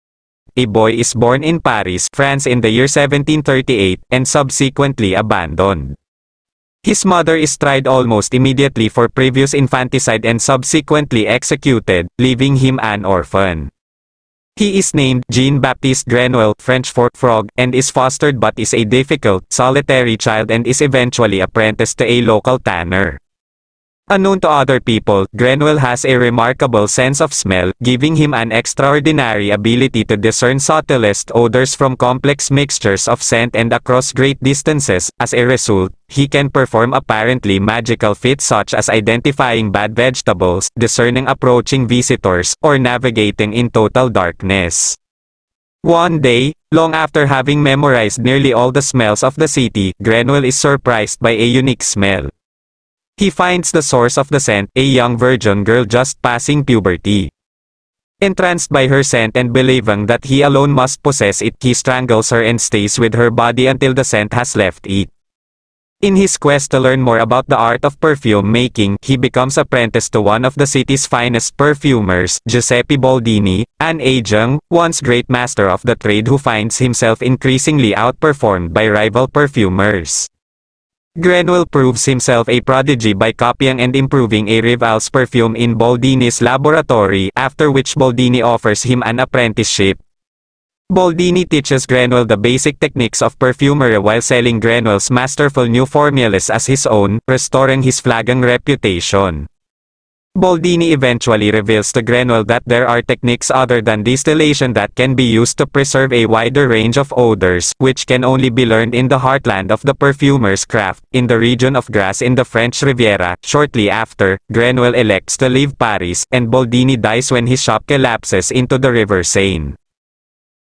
The Ferfume/the story of murderer/novel
the perfume
a boy is born in # Paris, France in the year 1738, and subsequently abandoned. His mother is tried almost immediately for previous infanticide and subsequently executed, leaving him an orphan. He is named "Jean-Baptiste Grenouille" (French for "frog") and is fostered but is a difficult, solitary child and is eventually # apprenticed to a local tanner. Unknown to other people, Grenouille has a remarkable sense of smell, giving him an extraordinary ability to discern subtlest odors from complex mixtures of scent and across great distances; as a result, he can perform apparently magical feats such as identifying bad vegetables, discerning approaching visitors, or navigating in total darkness.
One day, long after having memorized nearly all the smells of the city, Grenouille is surprised by a unique smell. He finds the source of the scent: a young virgin girl just passing puberty. Entranced by her scent and believing that he alone must possess it, he strangles her and stays with her body until the scent has left it. In his quest to learn more about the art of perfume-making, he becomes apprenticed to one of the city's finest perfumers, Giuseppe Baldini, an aging, once-great master of the trade who finds himself increasingly outperformed by rival perfumers. Grenouille proves himself a prodigy by copying and improving a rival's perfume in Baldini's laboratory, after which Baldini offers him an apprenticeship. Baldini teaches Grenouille the basic techniques of perfumery while selling Grenouille's masterful new formulas as his own, restoring his flagging reputation. Baldini eventually reveals to Grenouille that there are techniques other than distillation that can be used to preserve a wider range of odours, which can only be learned in the heartland of the perfumer's craft, in the region of Grasse in the French Riviera. Shortly after, Grenouille elects to leave Paris, and Baldini dies when his shop collapses into the river Seine.